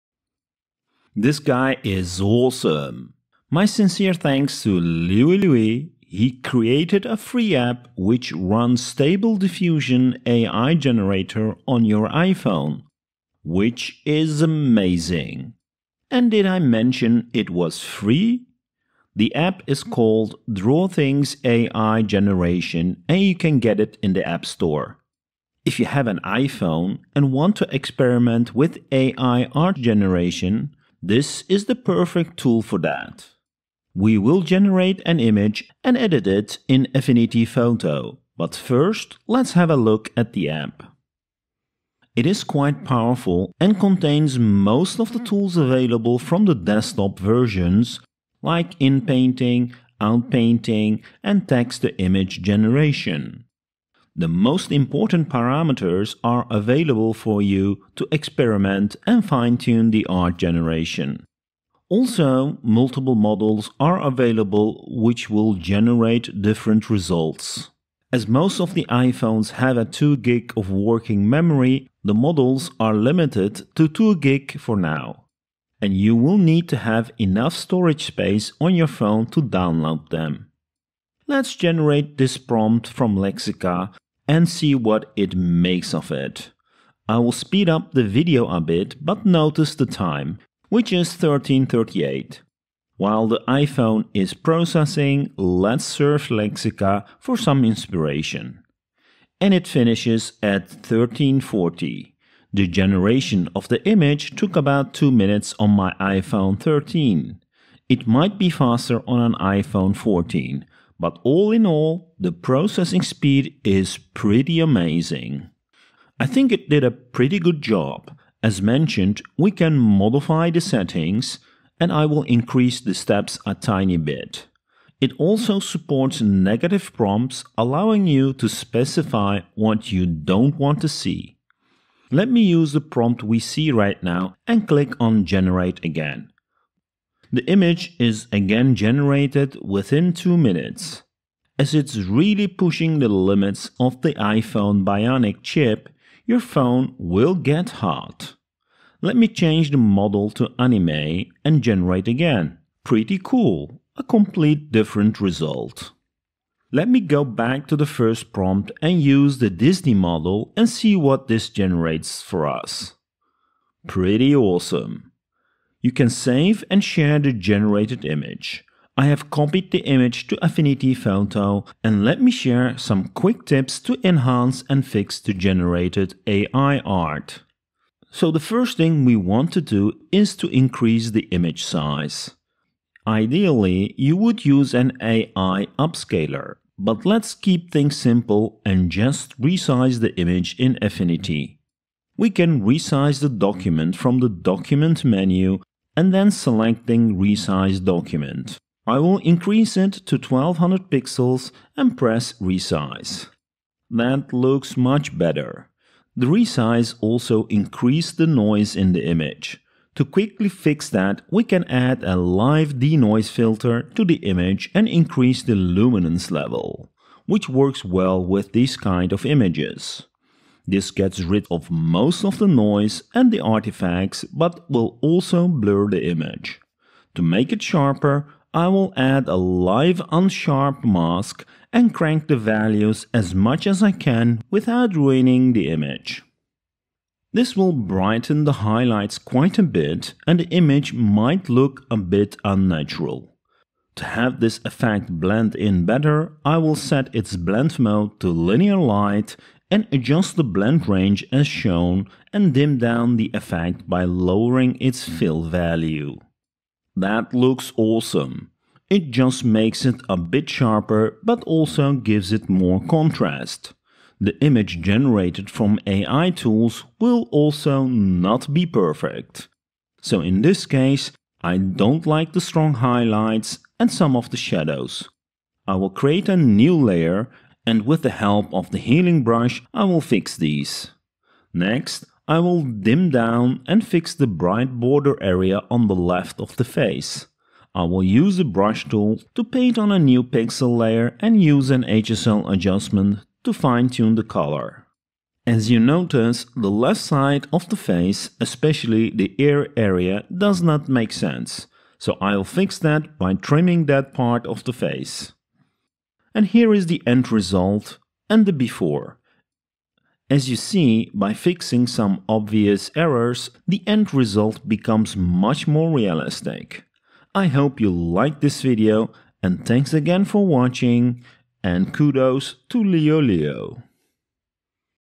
OMG! DRAW THINGS AI Generation for you iPhone using Stable Diffusion to create AI Art using a phone
This is just mind blowing. If you want to experiment with AI Art and do not have a powerful PC, you can now use your iPhone to generate images. In this video i will introduce the Draw Things: AI Generation app and give some tips how we can adjust the result in Affinity Photo 2.

The performance is quite well and so are the result. Definitely worth a try. Keep in mind you will need a newer IPhone (A12 bionic processor required).

If you are interested in upscaling the images with AI, i made a video on that too: